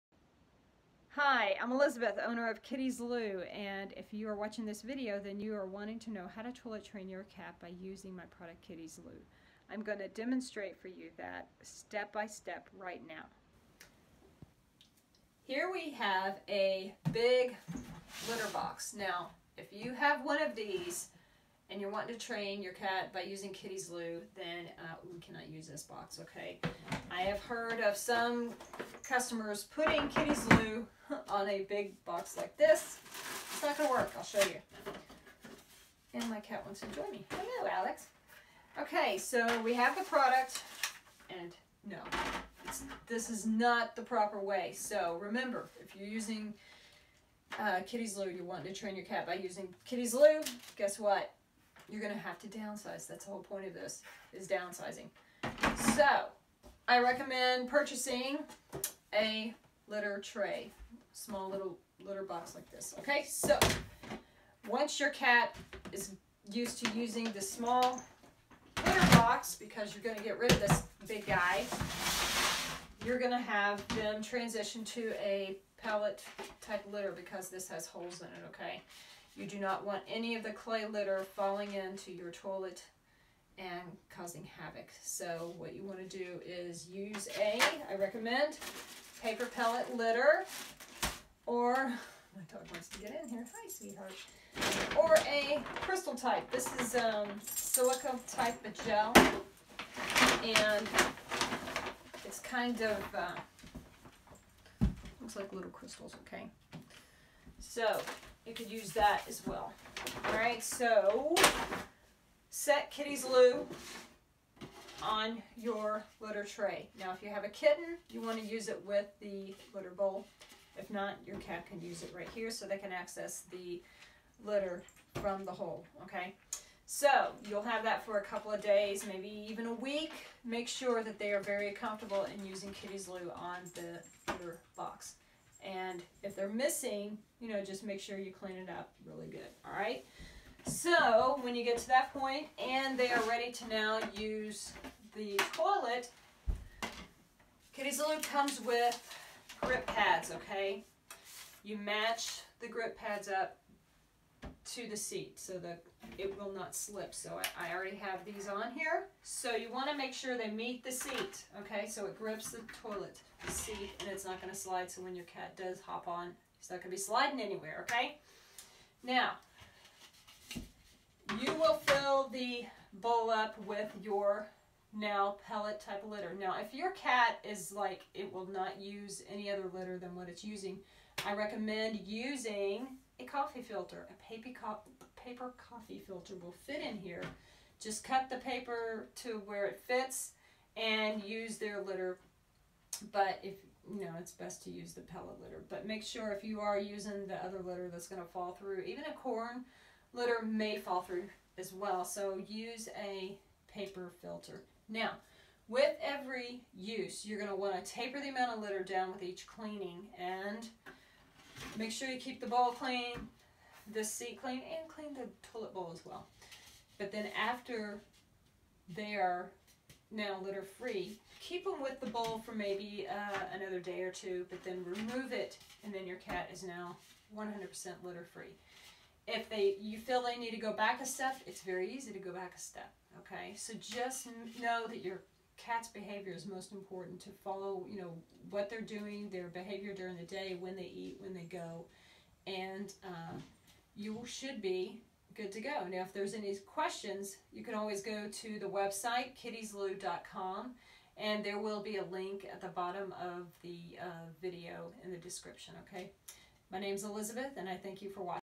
How to Toilet Train Your Cat with Kitty's Loo
If you want to toilet train your the right way, this video will show you how with Kitty's Loo.